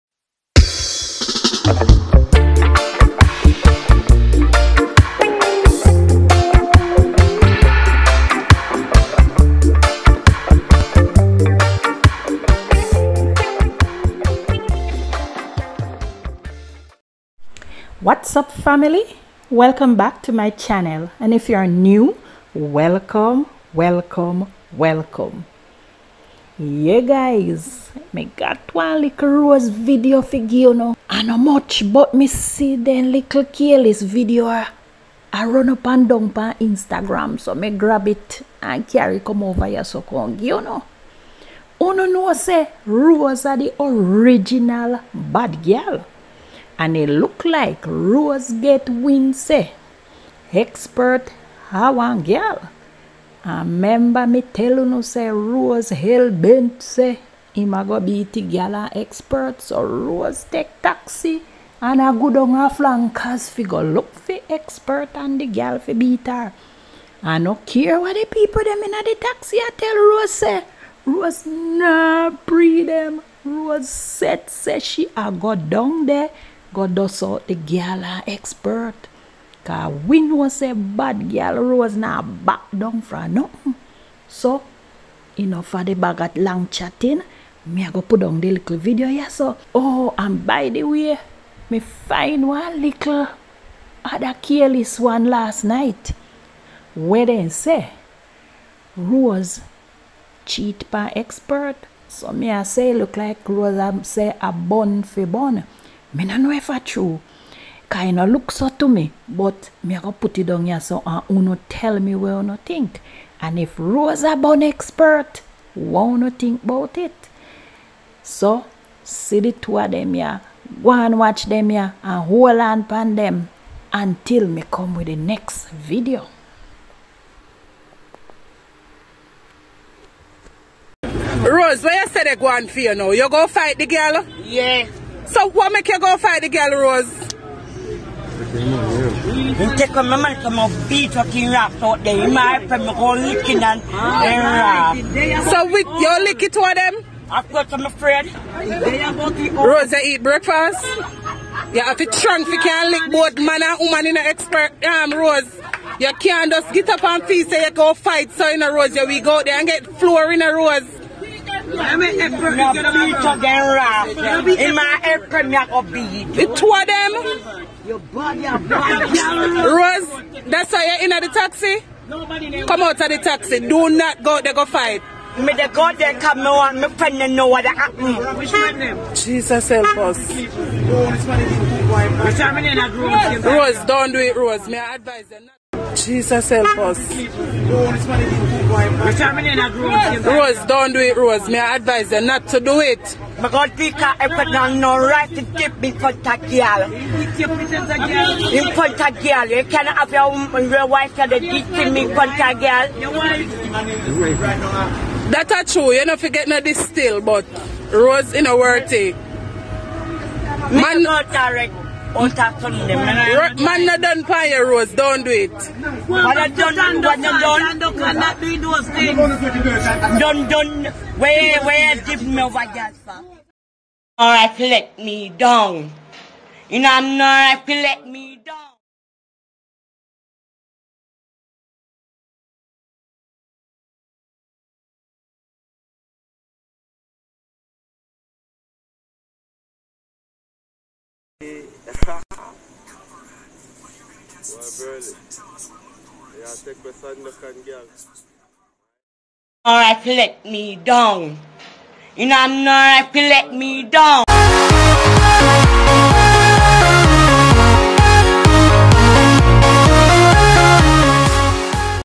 Rose S3x Tape Exposed | Bad Gal Rose at it Again - She a Go Be@t Expert and Him Side Chick
Bad Gal Rose on her way in a Taxi to confront Expert and his woman because she says Expert diss her in front of the woman.